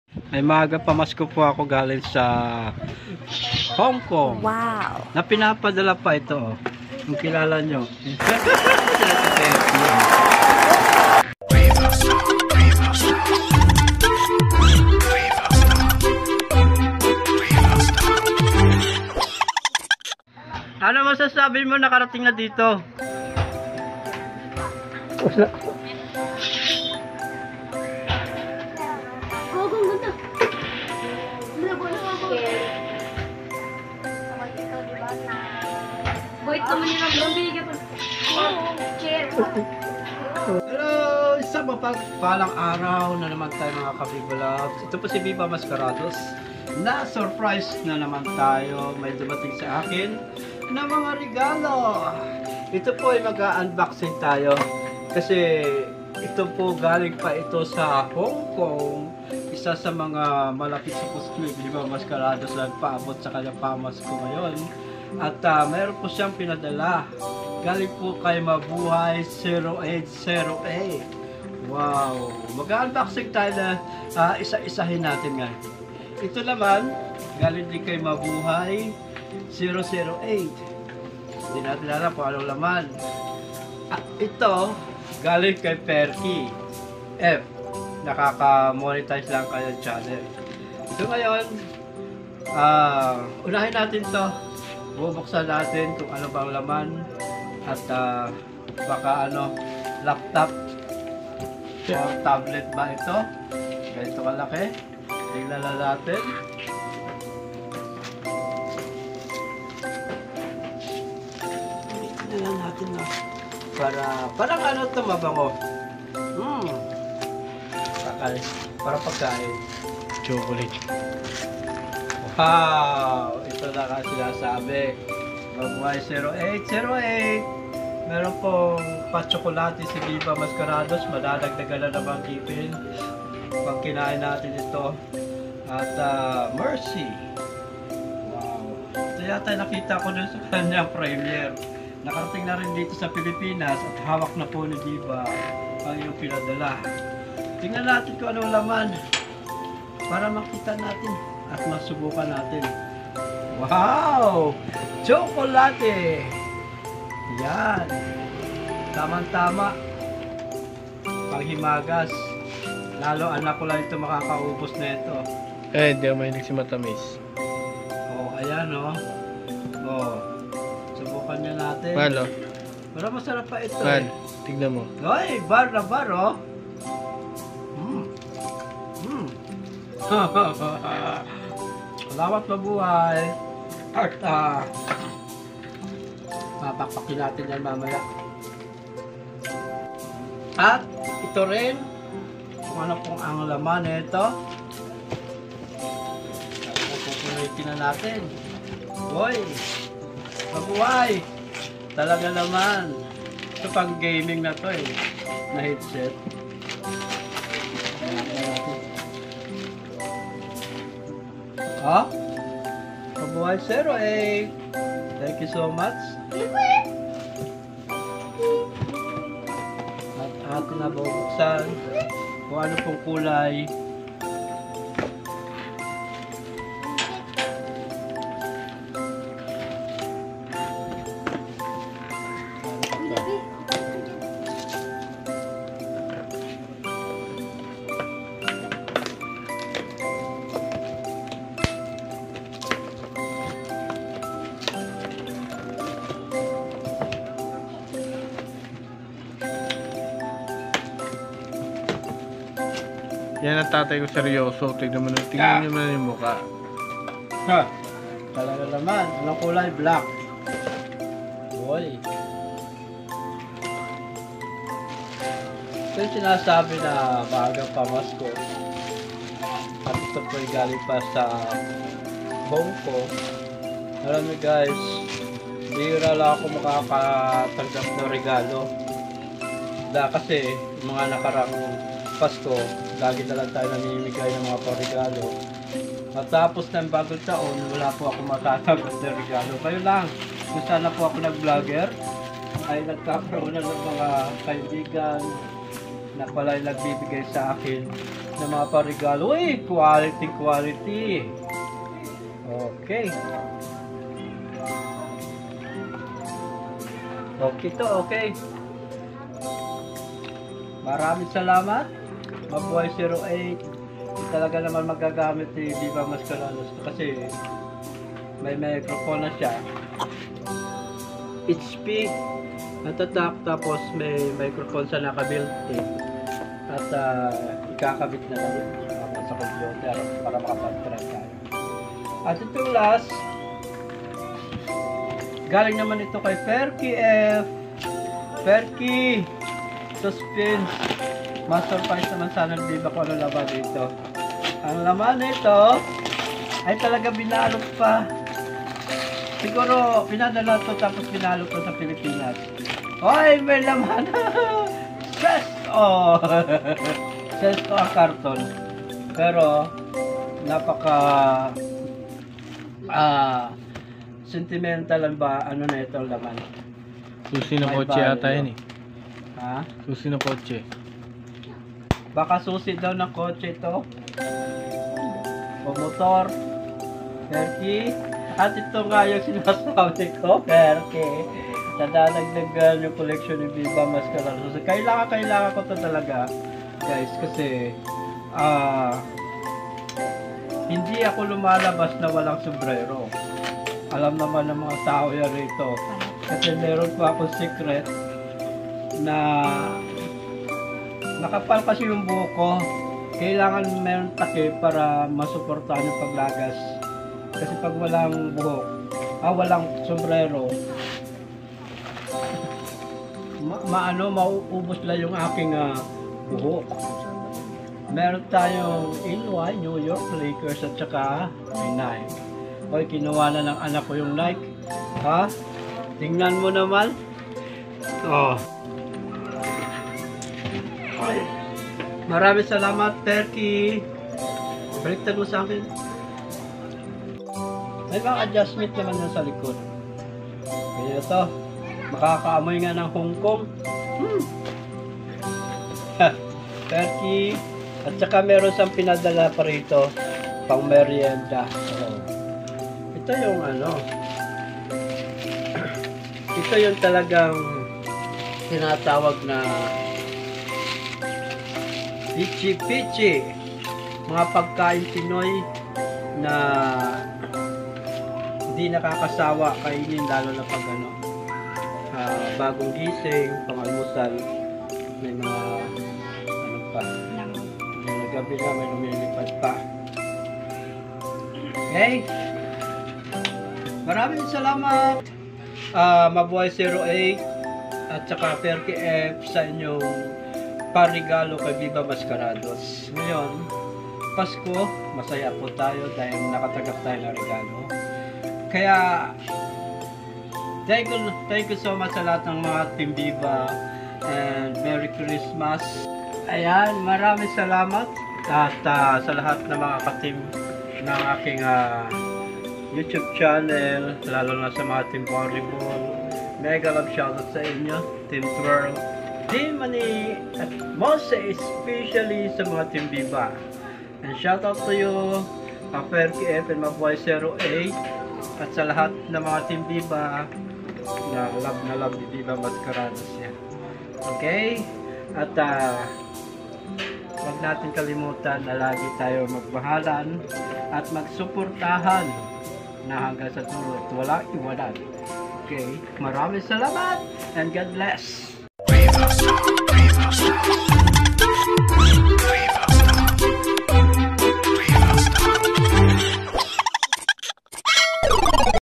NAUUSONG REGALO SA 2022 | REGALO SA PASKO
MGA NAUUSONG REGALO SA PASKO NA VERY USEFULL ISA SA MGA TIPS SA MGA NAGHAHANAP NA GUSTO MAG REGALO SA KANILANG MGA KAIBIGAN AT MAHAL SA BUHAY